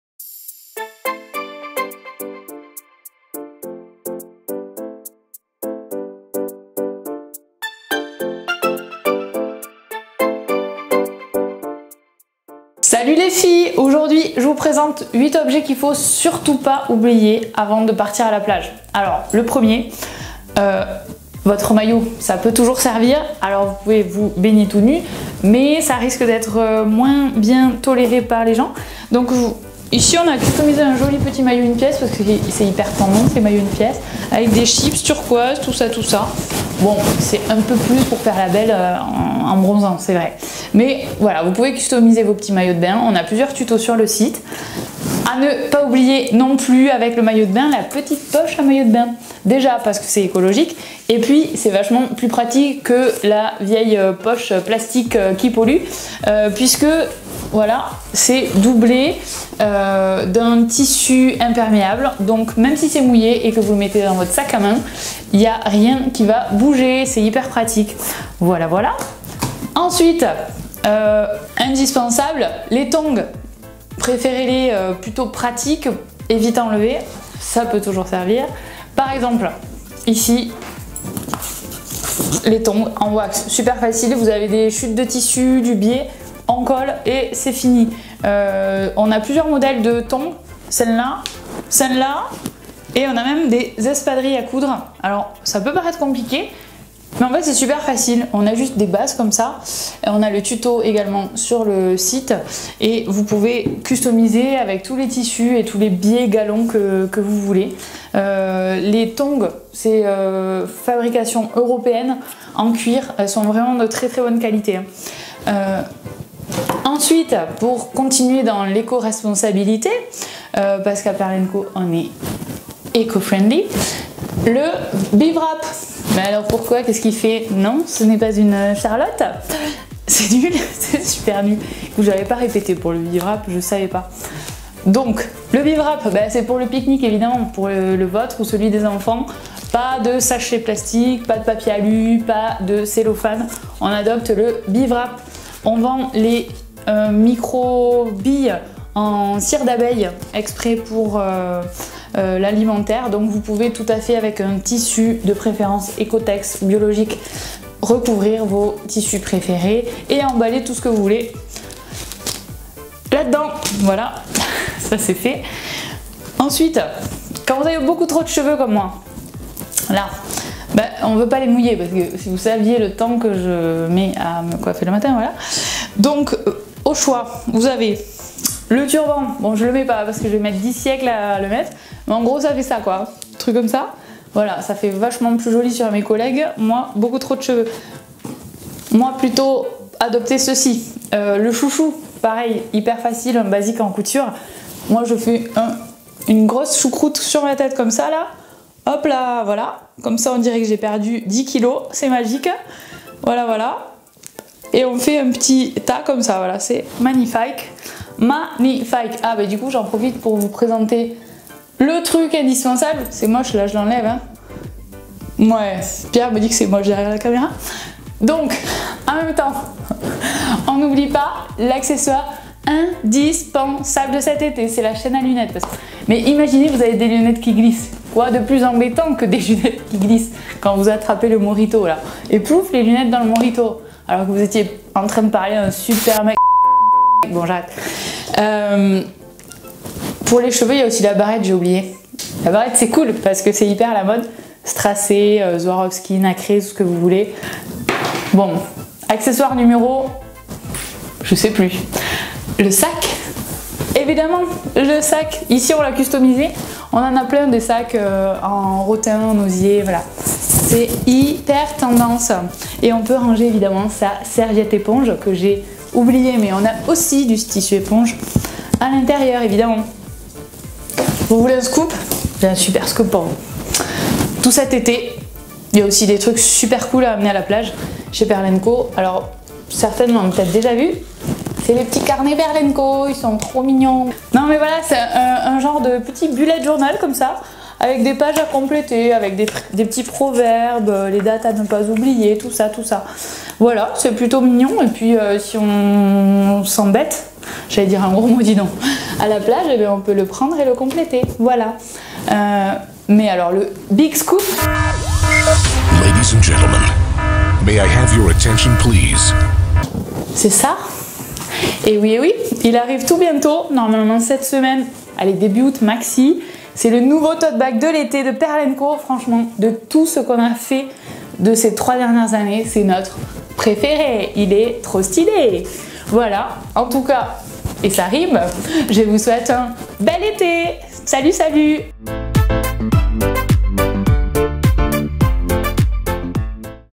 DIY | 10 ACTIVITES / TUTOS SPECIAL PLAGE & VACANCES A EMBARQUER DANS SA VALISE
Retrouvez tous les liens des tutos dans la description !

Comment réaliser un sac de plage ?
Comment customiser un maillot de bain une pièce ?

L'été et là !
Dans cette vidéo, nous avons le plaisir de vous présenter une petite sélection de quelques tutos à réaliser spécialement pour la plage.
On vous relooke de la tête au pied pour vos vacances, en passant par les lunettes, le maillot, les chaussures, etc... il y en a pour tout les goûts !
N'hésitez pas à nous dire en commentaire pour quels tutos vous avez craqué !